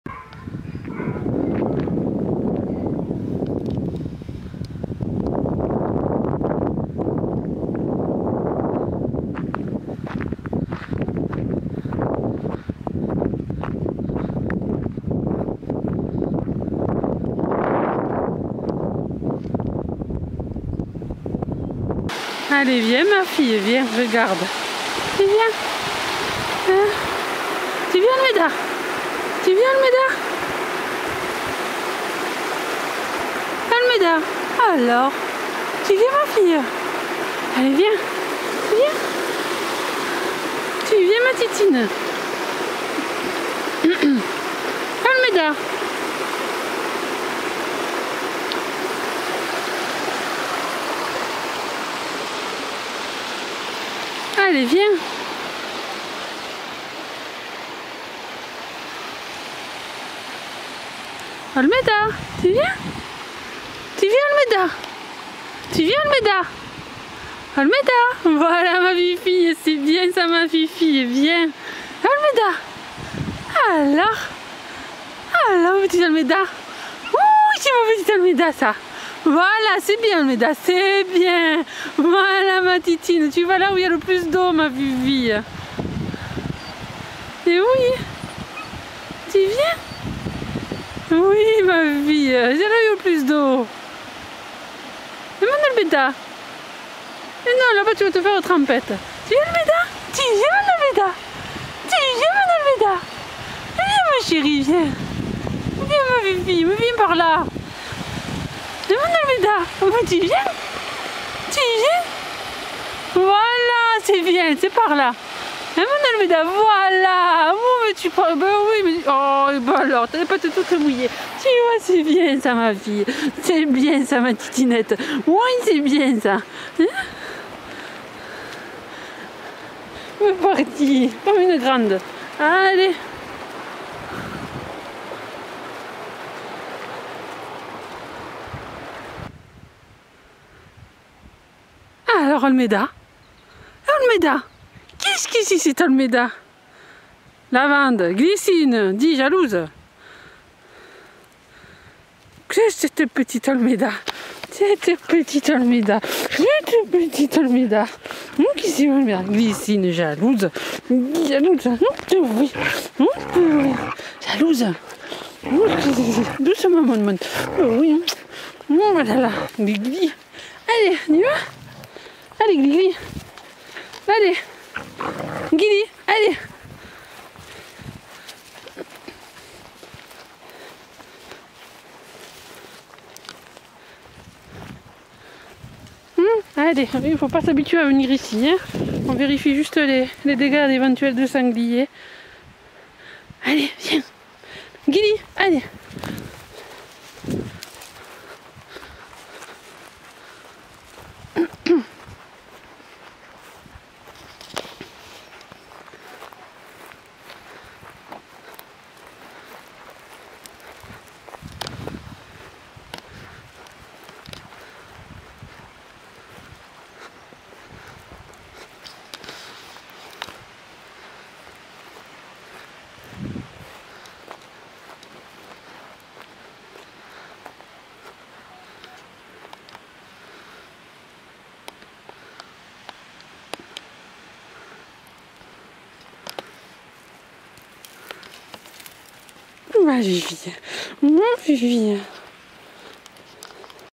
Chiens Border Collies non LOF des Tourelles - Notre nouvelle jeune chienne OLMEDA, en promenade !
Our new young dog OLMEDA, for a walk !

Notre ferme c'est la passion et l'élevage des
   - Chèvres miniatures
   - Chèvres alpines